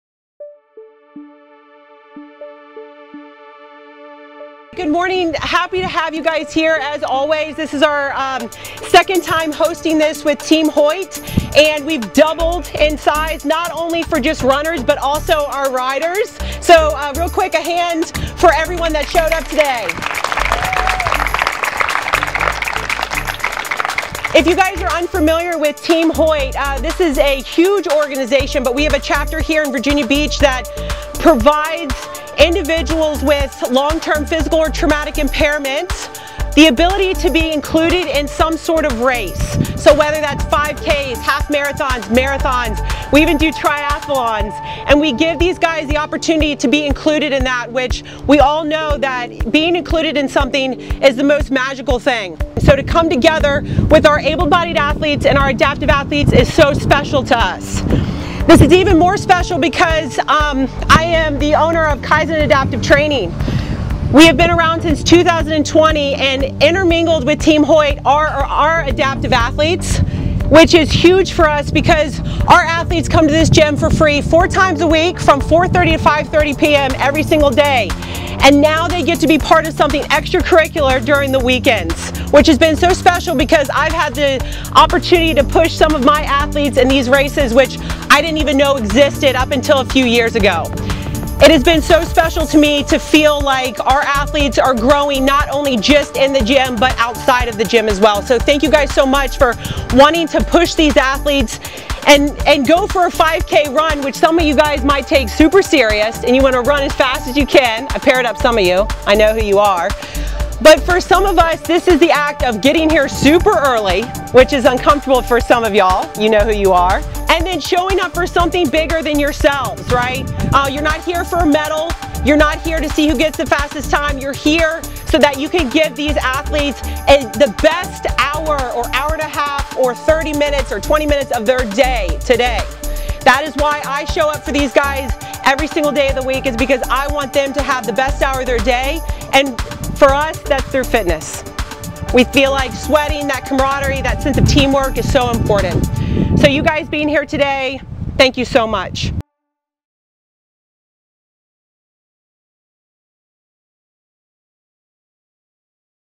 Kaizen Adaptive Training + Team Hoyt Adaptive 5K RUN!
Our 2nd annual adaptive athlete 5k race was a great opportunity to give back to our adaptive athletes! This event is for individuals with long-term physical or traumatic impairments as well as individuals with intellectual differences to be included in a race. Check out Kaizen Adaptive Training and Team Hoyt for opportunities to be involved.